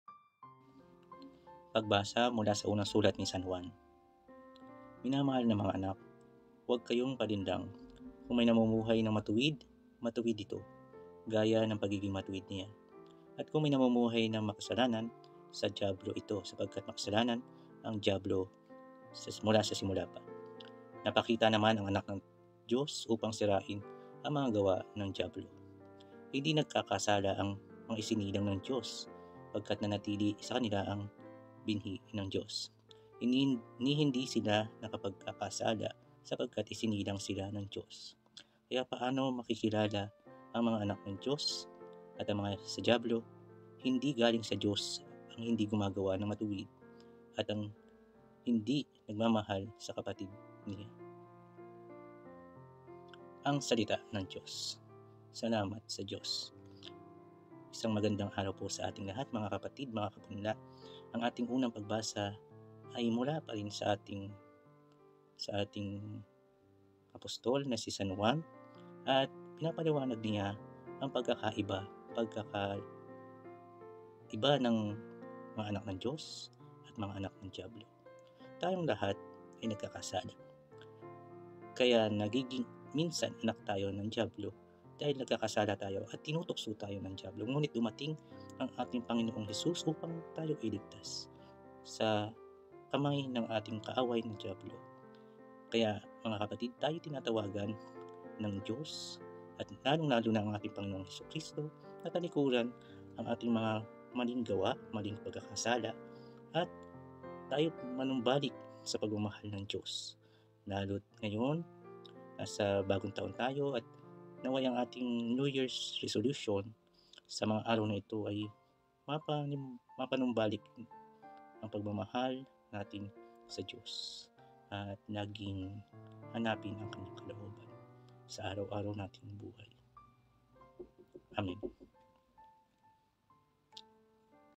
PUNLA Ep. 01-24 "Hindi sa Diyos, ang hindi gumagawa ng matuwid" 📖 FIRST READING: 1 Juan 3:7-10 🩵🙏
📖🙏 FIRST READING REFLECTIONS:
Bro Romeo Jarantilla Jr